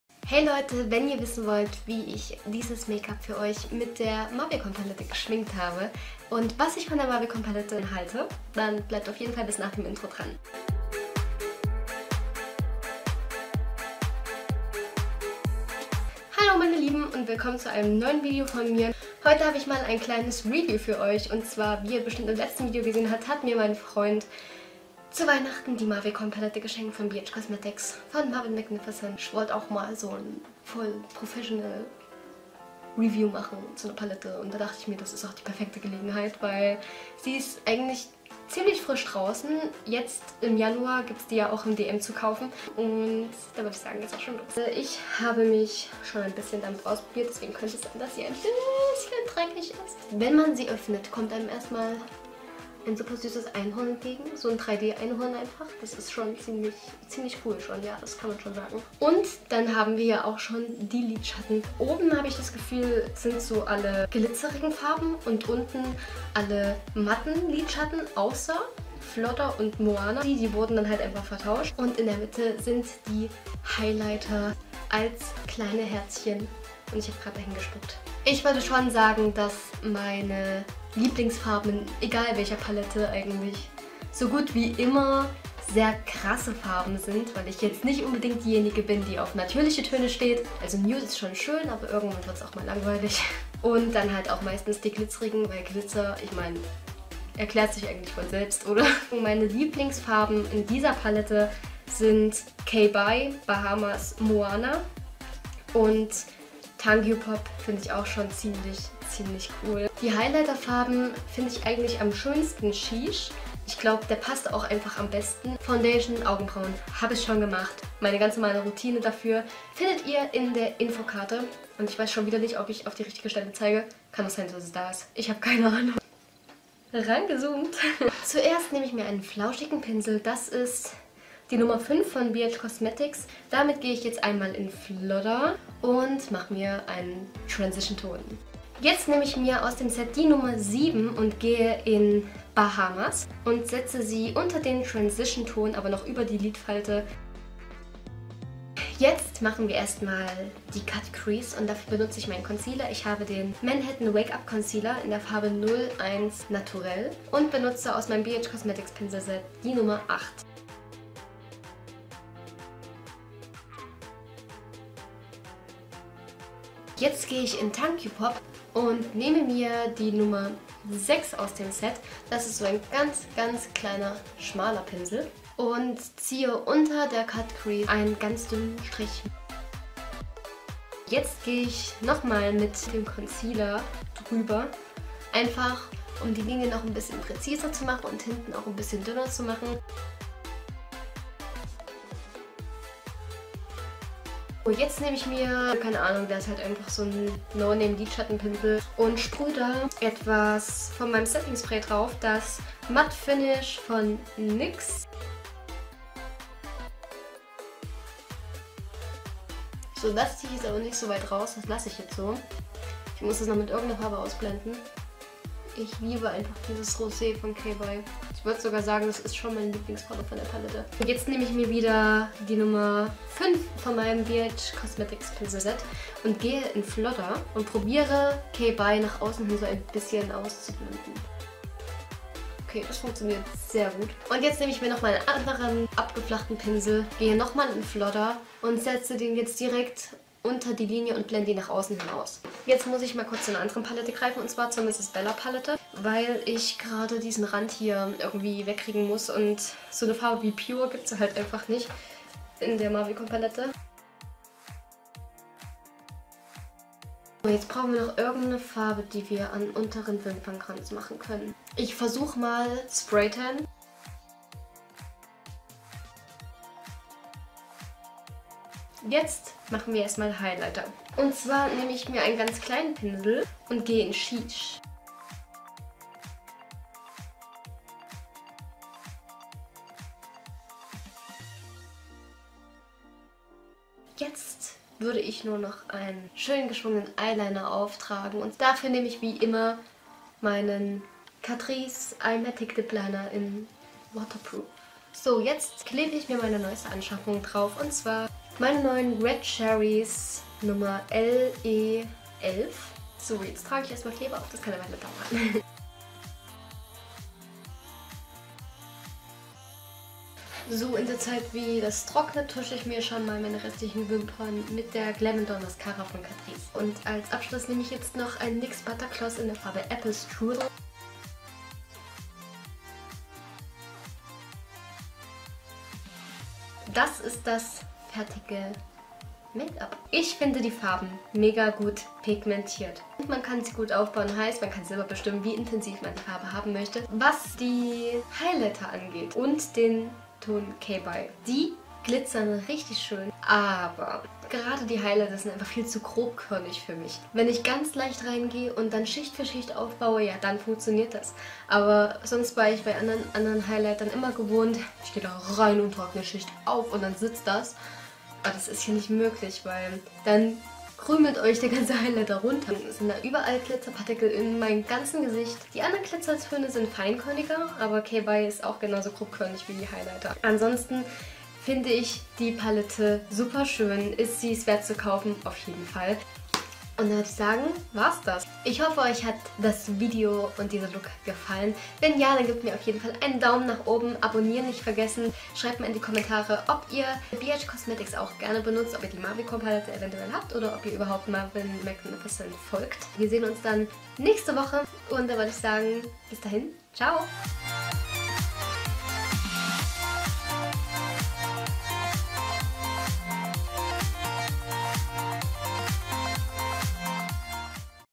MARVYCORN Palette 🦄 - Top oder Flop?! | Steffi Versatile
Heute habe ich euch mal die neue Marvycorn Palette von BhCosmetics und Marvyn Macnificent reviewt - Viel Spaß beim anschauen! 👍